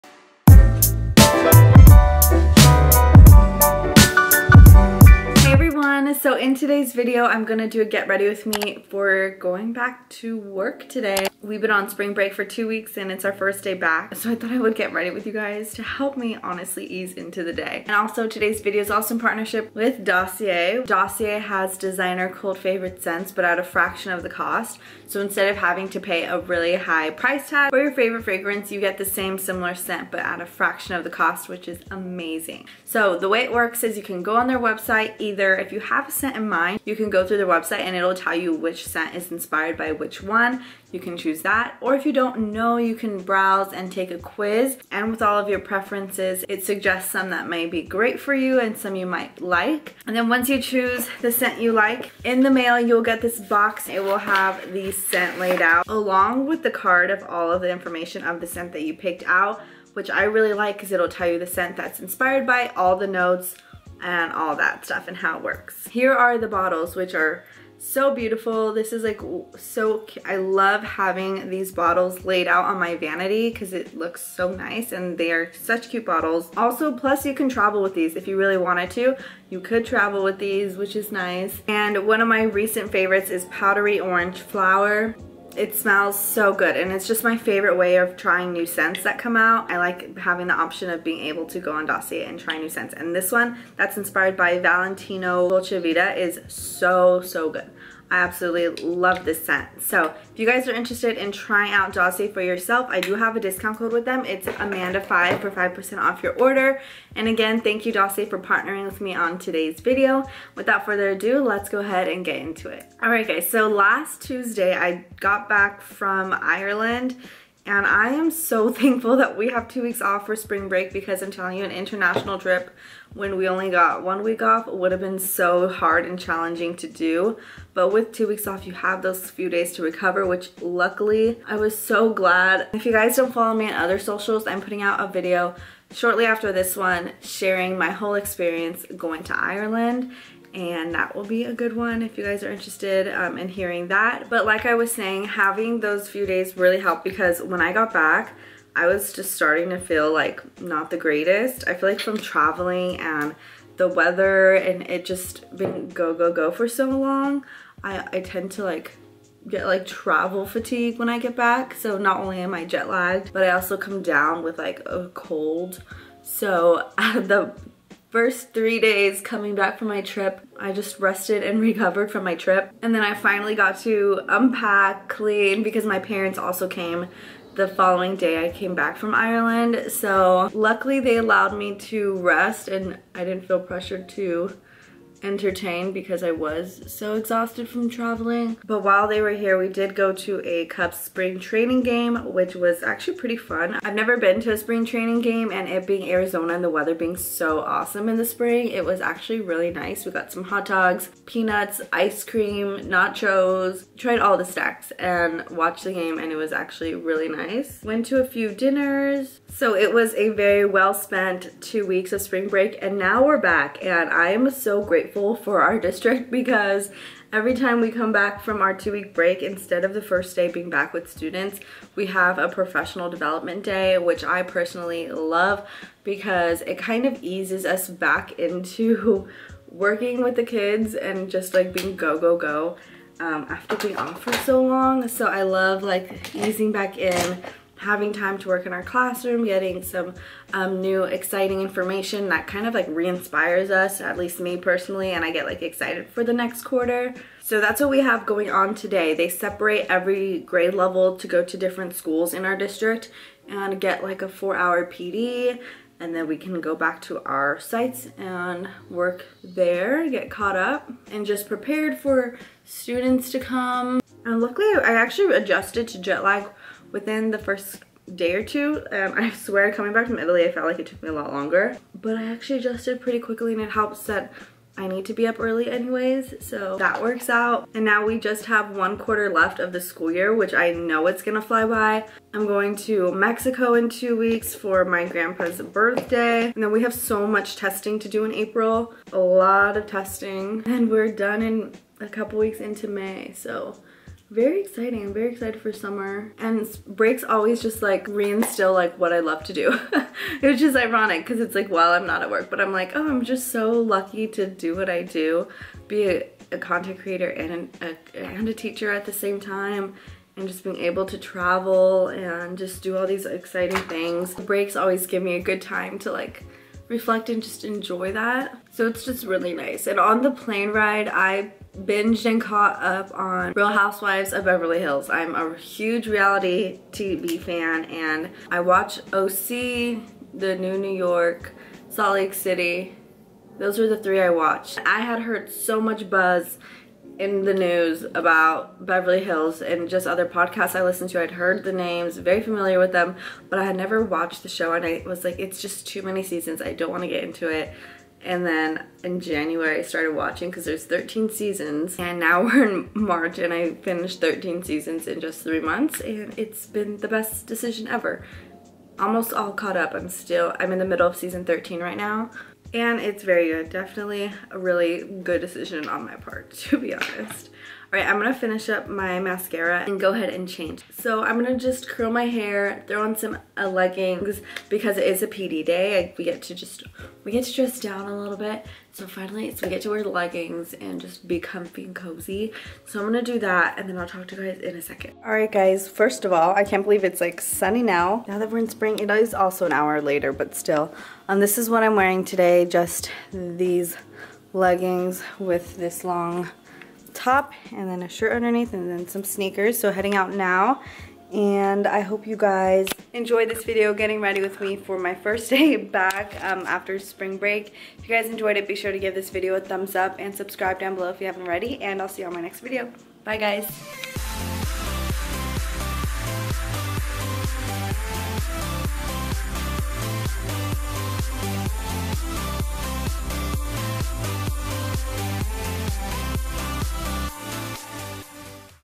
FIRST DAY BACK AFTER SPRING BREAK!! // GET READY WITH ME FT. DOSSIER
Hey guys! Today's video is a get ready with me for work after spring break!!

my classroom wishlist!

SD card